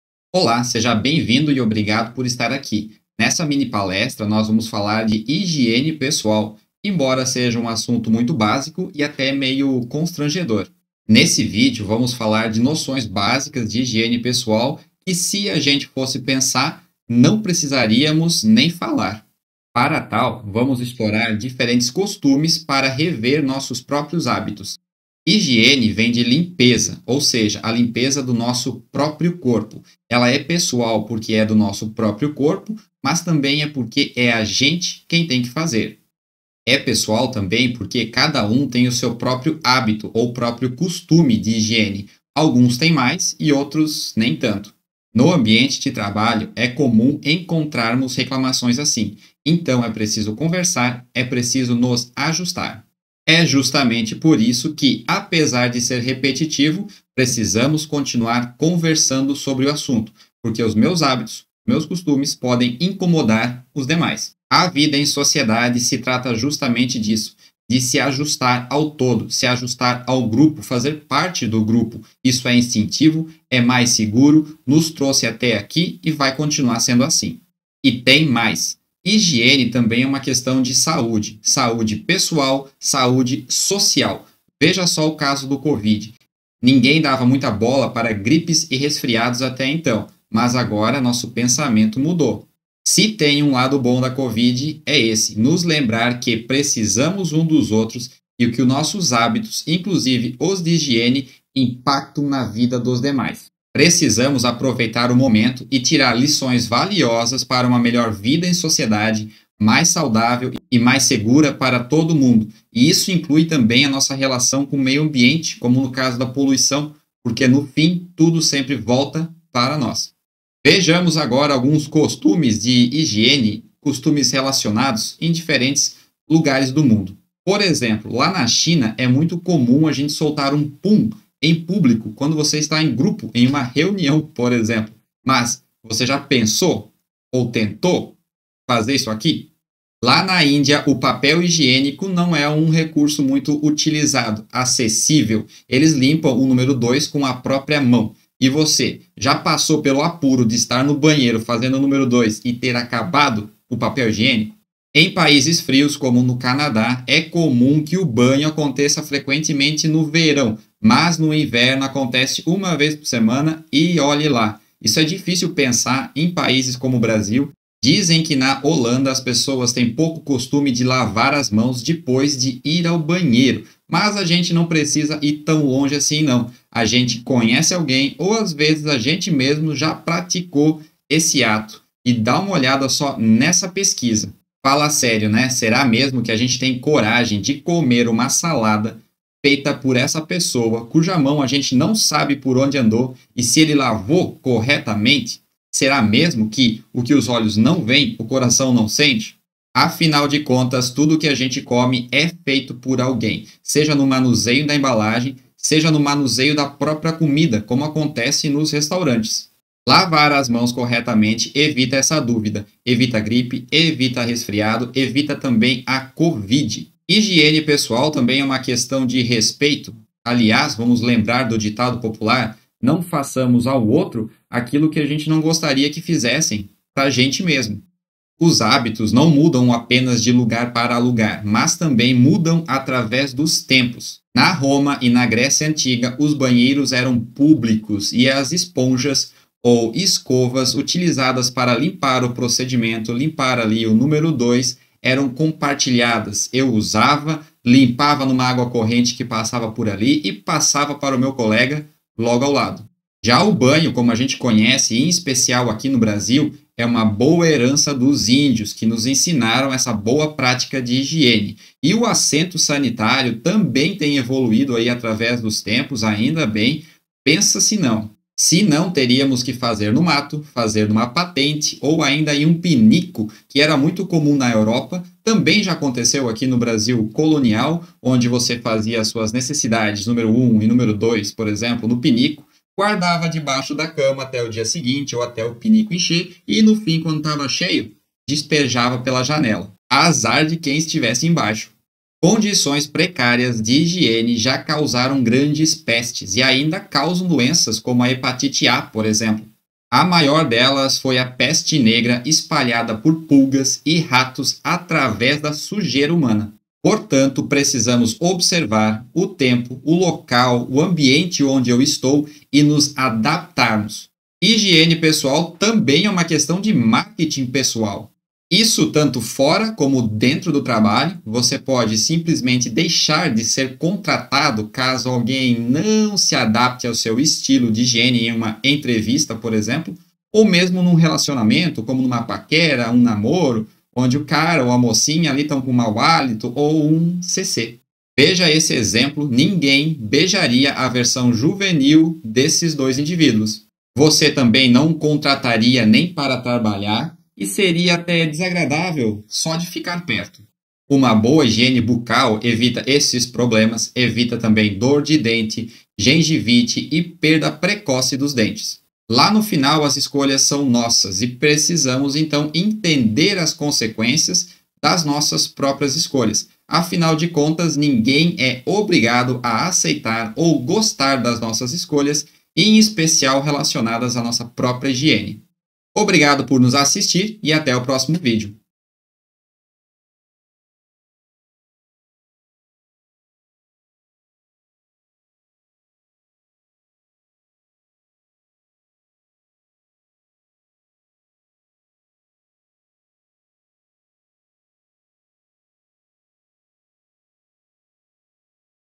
Higiene pessoal | Mini palestra gratuita | SIPAT Online
Nessa mini palestra online, temos a higiene pessoal no trabalho no centro da conversa como uma forma de adequação social e saúde. Bom tema para complementar suas iniciativas de saúde e segurança do trabalho. Conheça essa e outras opções para sua SIPAT Online em nosso site: